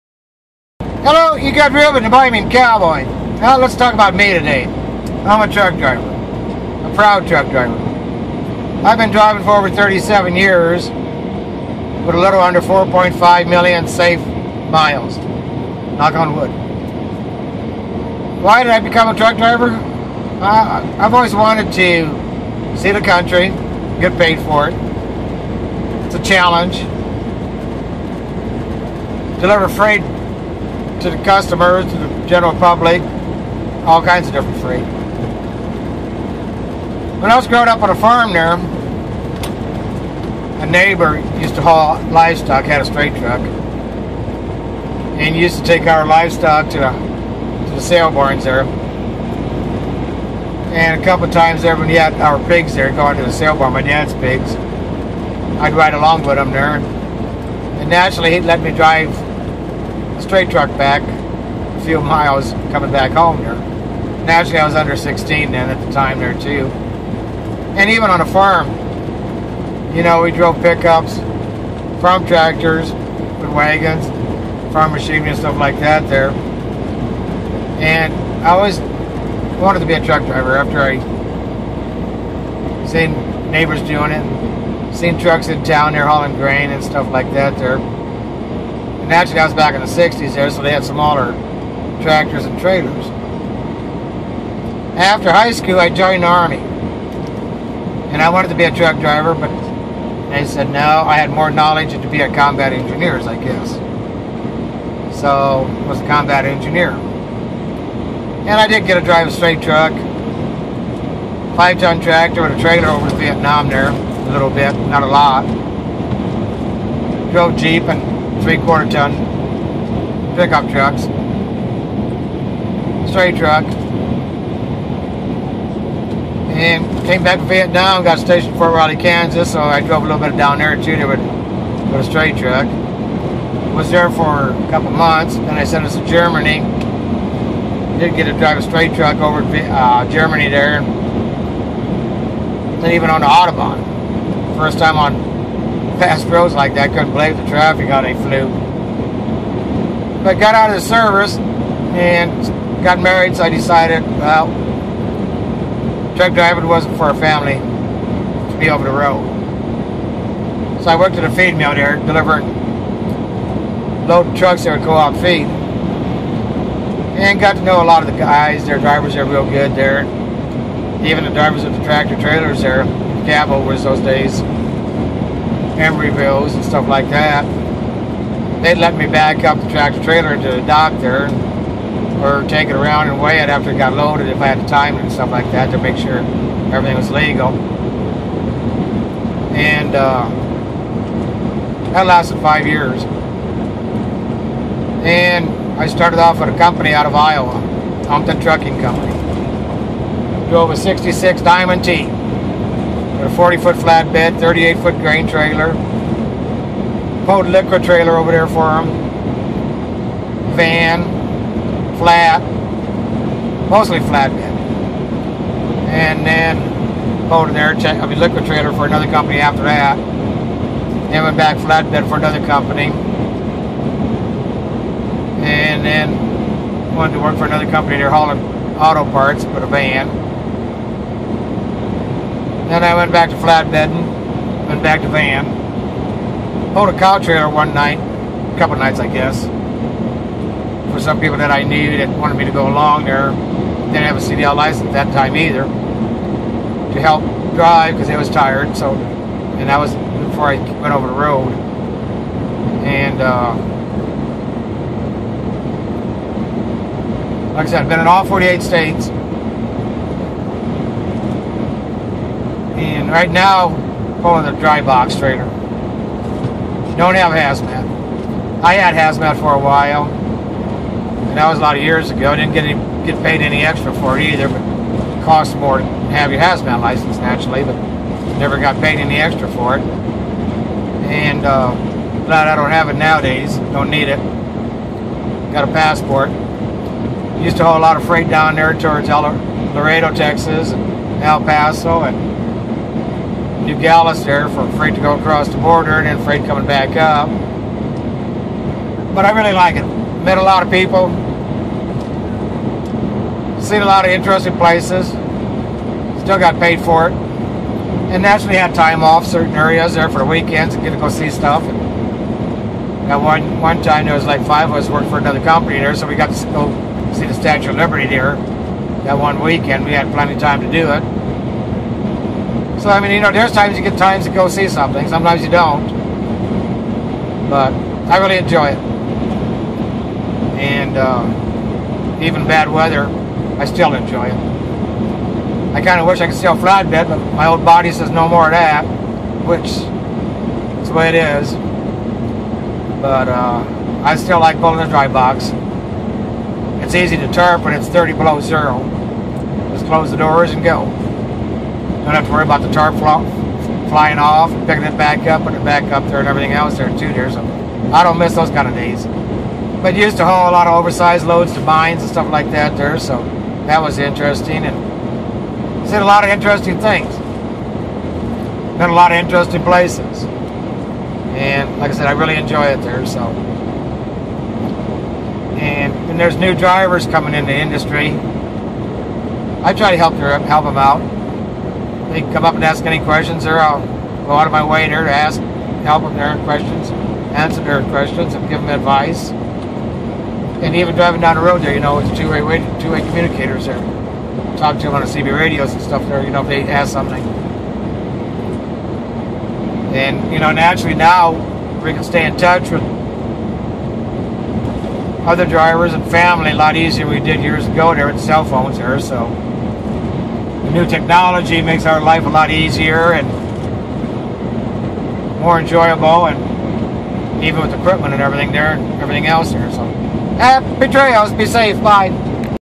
Why & how did I become a truck driver
Been driving over 37 years & over four millon safe miles, drove on the farm and Vietnam first, worked in feed mill, pulled flatbed, tank, hopper, cow wagon, drop deck, stretch flatbed,van,48 states